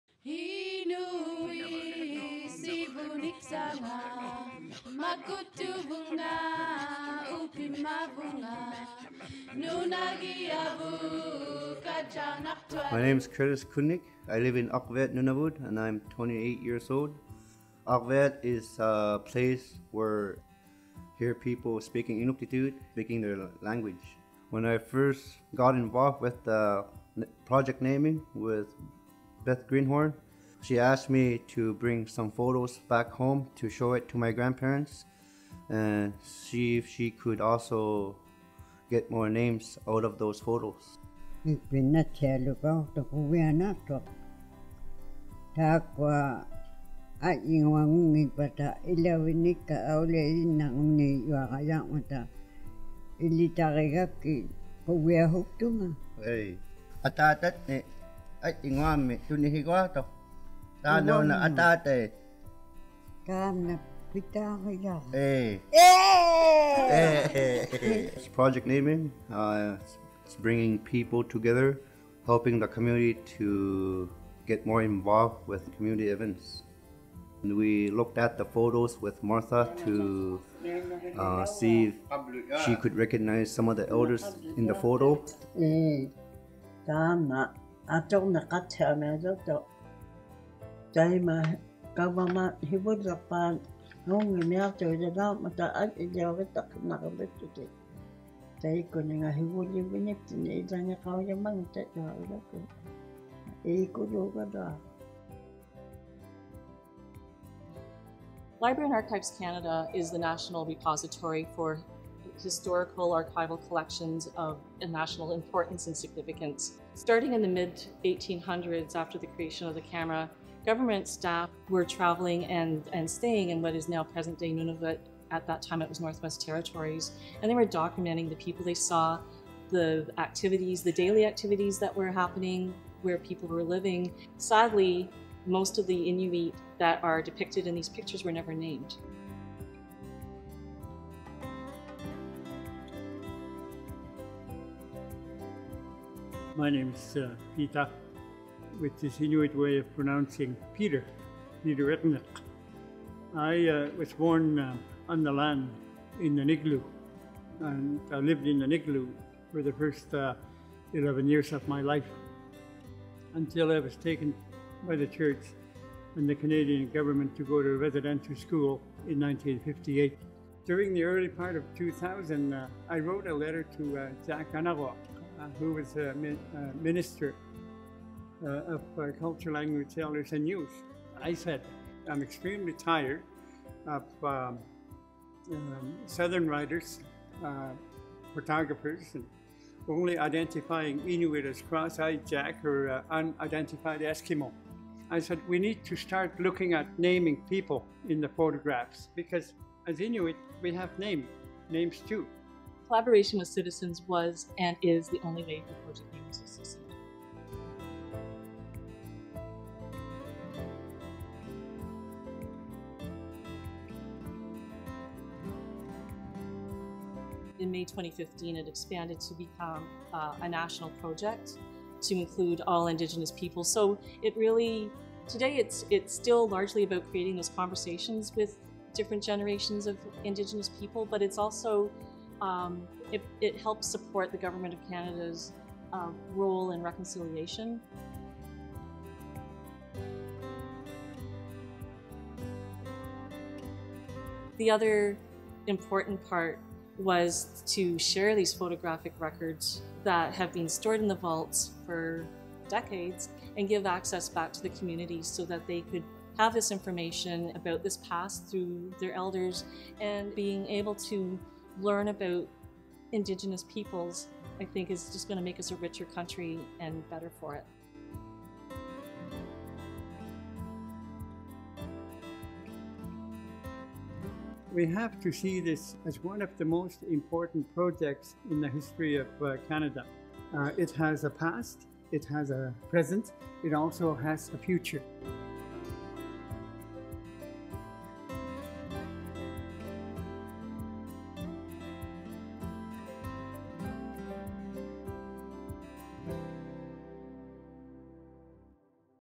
Project Naming - Every Picture Tells a Story
In 2002, the photo identification initiative Project Naming was born—a ground-breaking partnership between our organization, the Government of Nunavut, and Nunavut Sivuniksavut. For nearly two decades, Project Naming has forged meaningful connections with Indigenous communities by decolonizing our archival collection and leveraging community knowledge to improve descriptions.